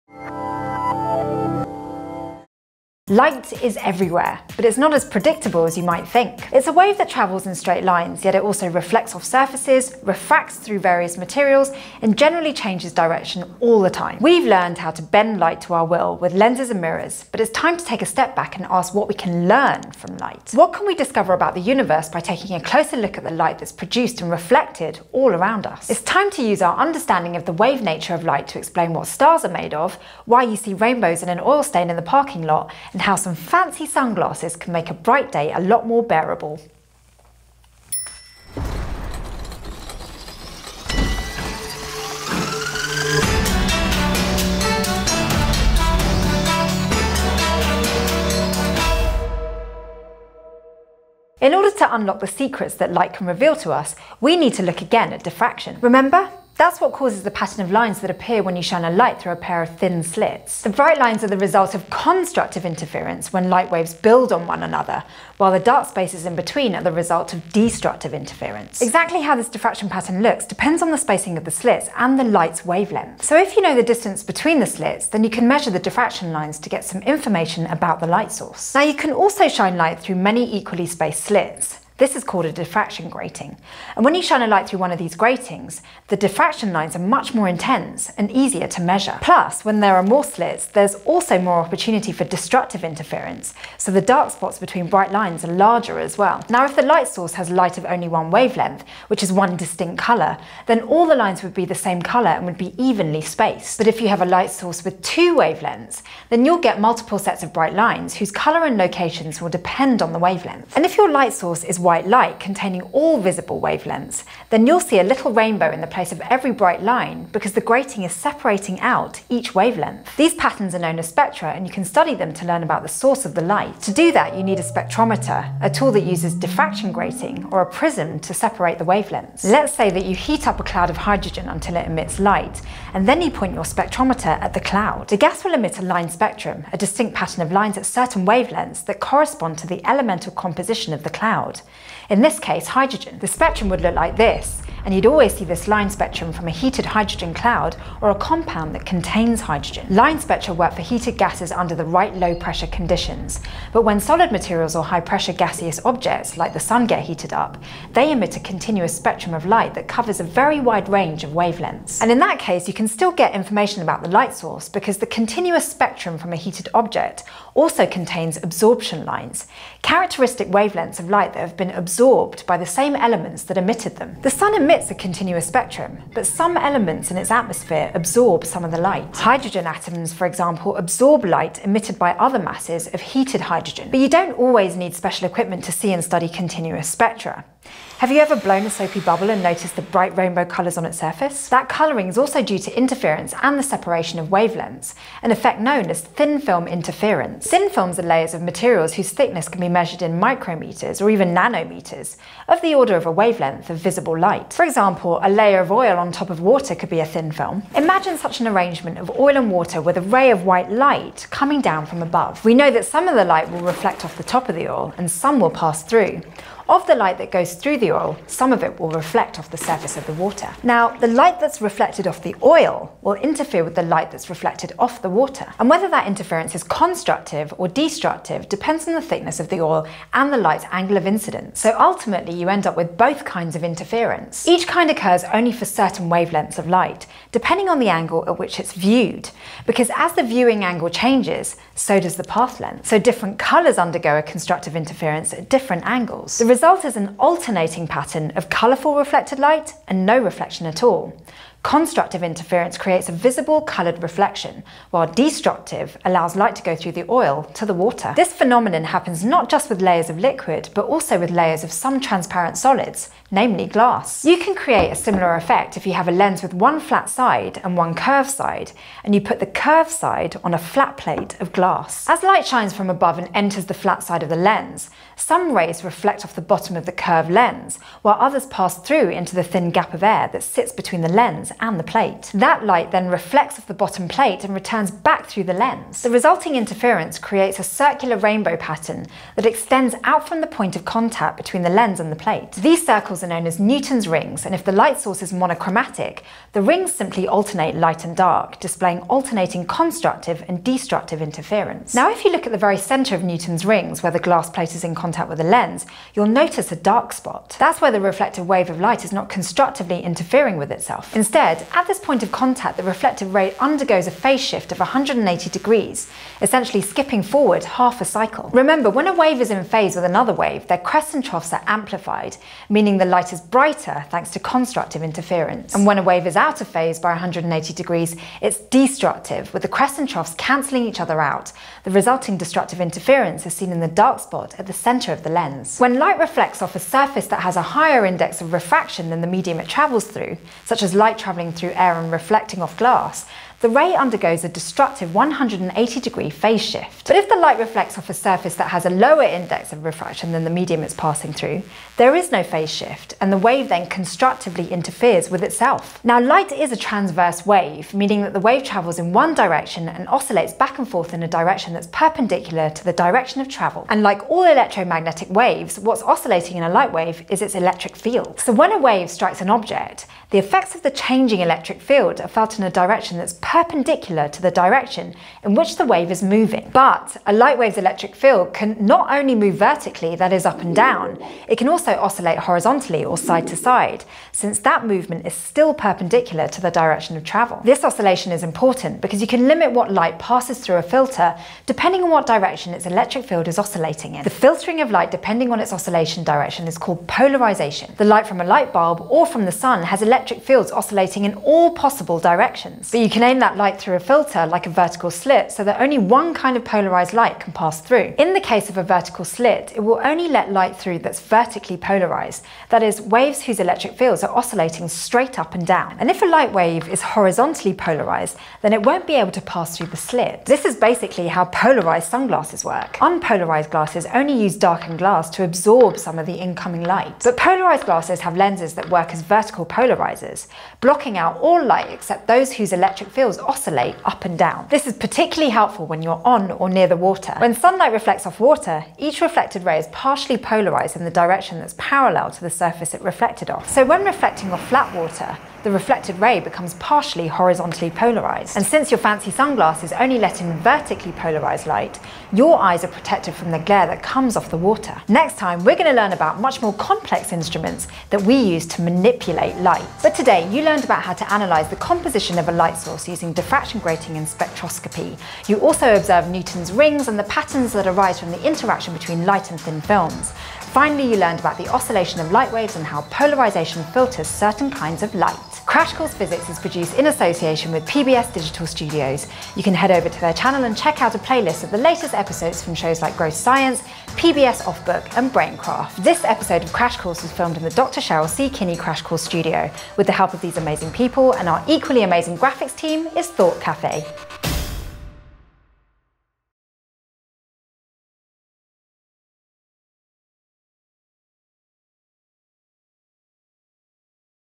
Spectra Interference: Crash Course Physics #40
Light is everywhere … but it’s not as predictable as you might think. It’s a wave that travels in straight lines, yet it also reflects off of surfaces, refracts through various materials, and generally changes direction all the time! We’ve learned how to bend light to our will, with lenses and mirrors, but it’s time to take a step back and ask what we can LEARN from light.